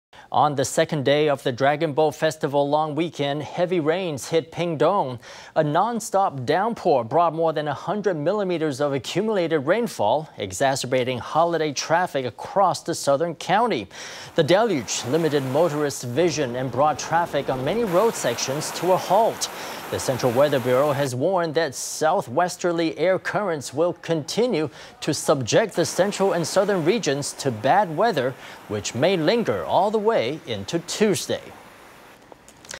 Heavy Rain in Pingtung
Heavy rains hit Pingtung today, the second day of the Dragon Boat Festival long weekend. Accumulated rainfall broke the 100-millimeter mark, affecting traffic across the county.  Increased holiday road traffic was made worse by a non-stop downpour, which limited motorists’ vision and slowed or halted traffic on many road sections. The Central Weather Bureau has warned that southwesterly air currents will continue to bring bad weather to central and southern Taiwan that may linger all the way into Tuesday.